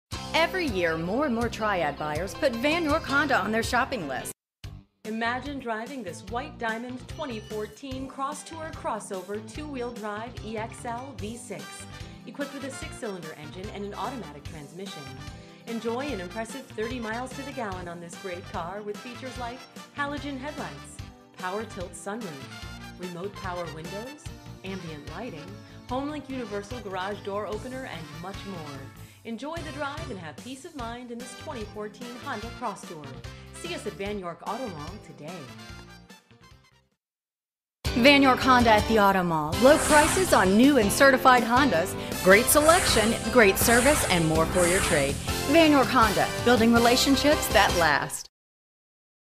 Used 2014 Honda Crosstour 2WD EXL V6 Greensboro, Winston Salem High Point, Kernersville NC Vann
New 2014 Honda Crosstour 2WD EXL V6 Greensboro, Winston Salem High Point, Kernersville NC Vann, Imagine driving this White Diamond Pearl Honda Crosstour Crossover, equipped with a 6 Cylinder engine and an automatic transmission. Enjoy an impressive 30 miles to the gallon on this great car with features like Halogen Headlights, power Tilt sunroof, Remote Operation Power Windows, Ambient Lighting, Homelink Universal Garage Door Opener, Auto-Dimming Inside Rearview Mirror, Heated Exterior Mirrors, Power Adjustable Lumbar Driver Seat, Remote Anti-Theft Alarm System, Glass, Rear View Camera, Remote Operation, Memory Settings For Exterior Mirrors, power One-Touch Tilt Open And Close Sunroof, Electronic Brakeforce Distribution, Front Fog Lights, Leather Upholstery, Power Driver and Passenger Seating, 6 Disc Cd Player In Dash, Heated Driver and Passenger Seating, and much more. Enjoy the drive and have peace of mind in this Honda Crosstour. See us at Vann York Auto Mall today!VIN:5J6TF1H53EL000690

422 Eastchester Dr.
High Point, North Carolina 27262